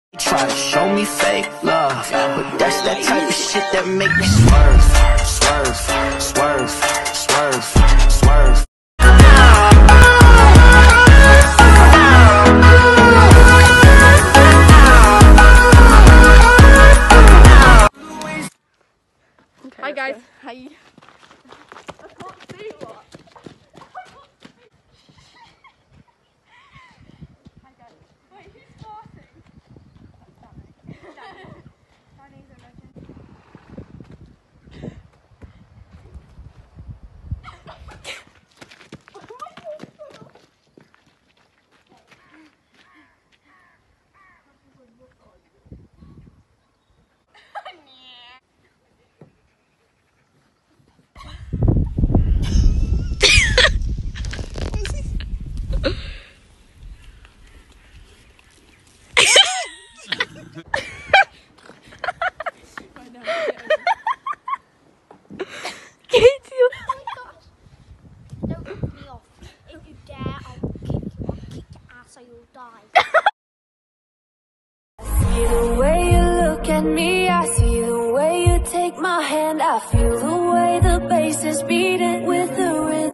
SEEING TYSON AND FINN FOR THE FIRST TIME IN A WHILE
So today zara and ruby went to see Tyson and Finn for a couple of hours 😘 ee also had the funniest time
We tried to make a vlog but in the end it turned out to be a few of our favourite funny clips! 💜
Make sure you follow @equinezara_x and @rubyrides_x on Instagram xx